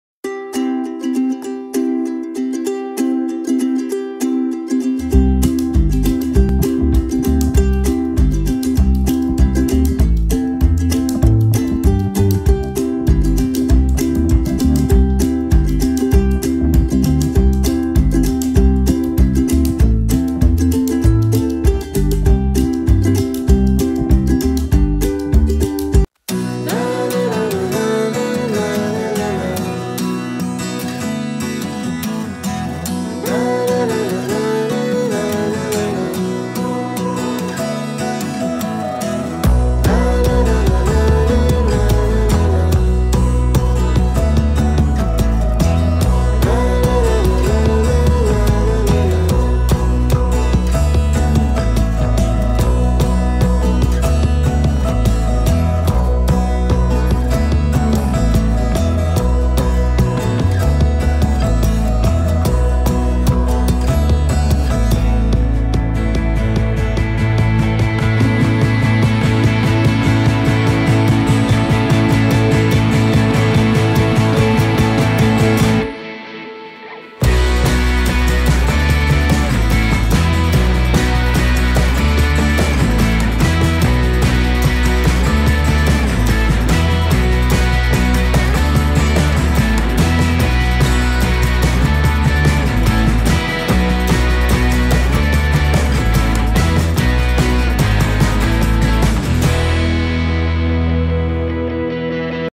Baby Funny 😂 Reaction During Meal Time | Baby Enjoying its Eating Time Lovely Toshi How to Feed baby
BABY FUNNY AND NAUGHTY MEAL TIME

Toshi yaha pr apko bahut sari majedar and mnoranjak activities karke dikhane walihai. Kids Toys Unboxing and Many more things.